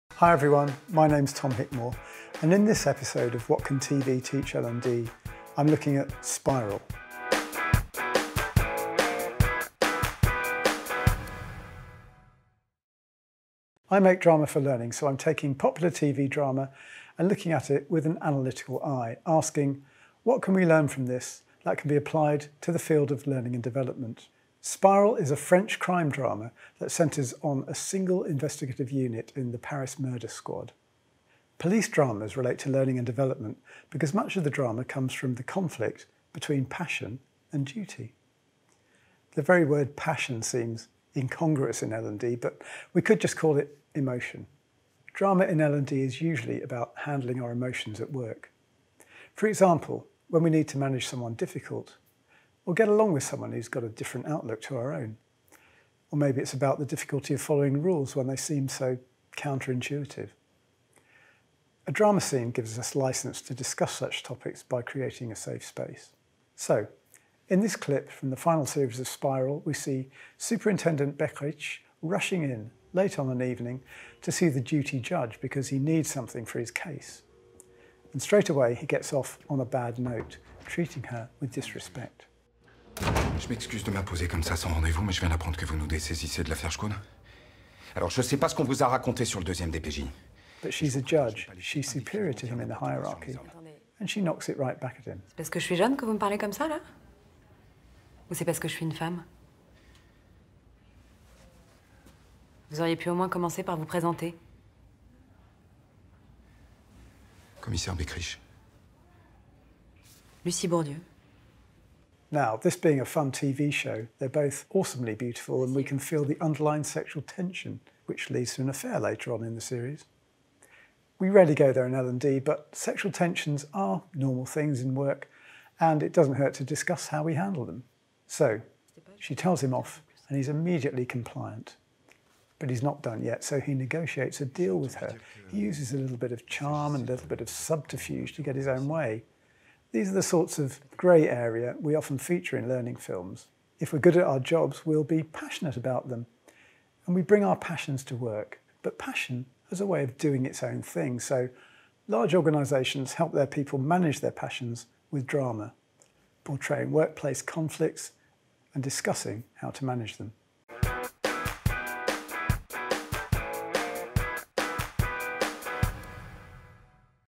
What Can TV Teach L&D: Ep 4 - Spiral
Veteran video director for the learning and development sector, Tom Hickmore, analyses popular TV asking "What can we learn from this that can be applied in the field of learning and development?"

In this episode Tom looks at Spiral. He talks about how emotional workplace situations can be shown on screen and the effect it can have on an audience.